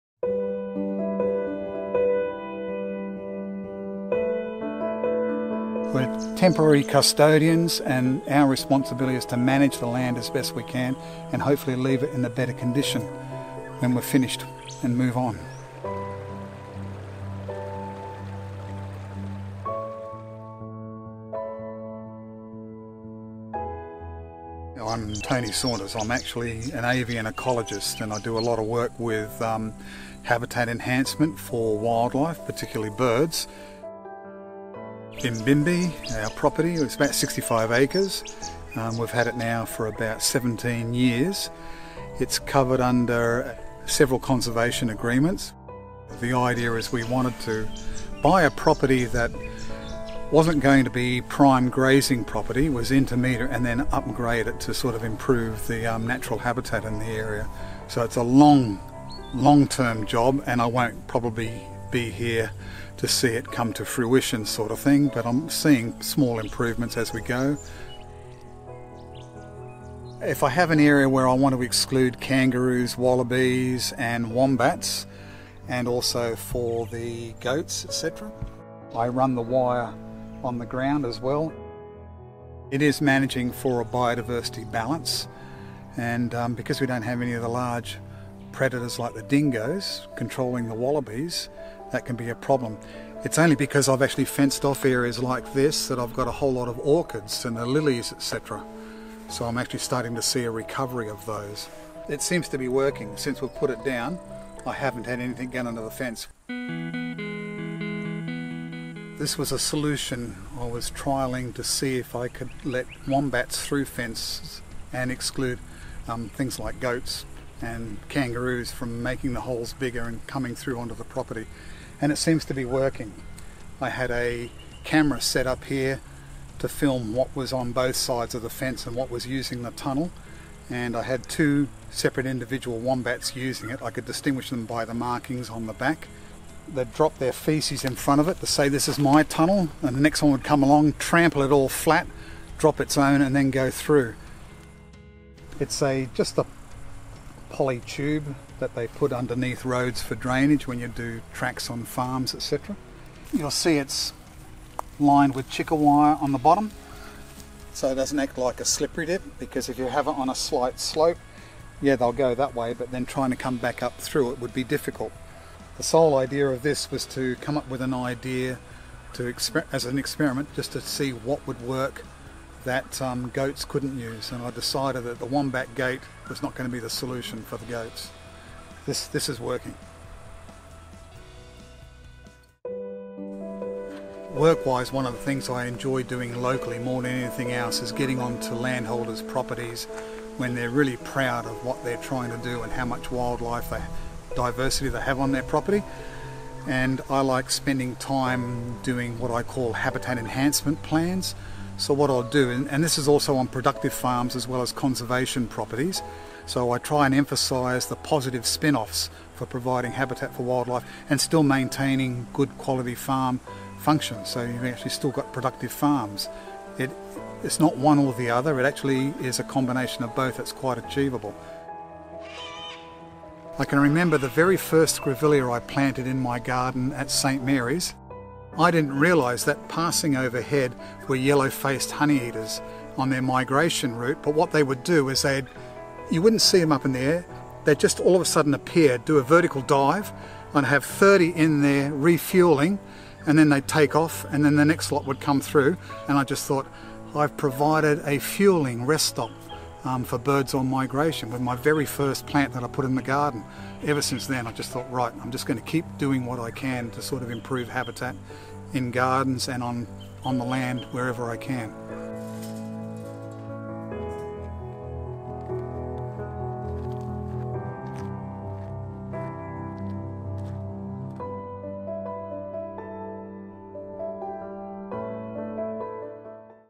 Remembering Tony Saunders, Upper Lachlan Shire
K2W Link honours the memory of Tony Saunders. He was so pivotal in enhancing the K2W Wildlife corridor and the surrounding environment. Tony volunteered so much of his time to enhancing the environment and sharing his vast knowledge of habitat for wildlife. Video by Richard Snashall.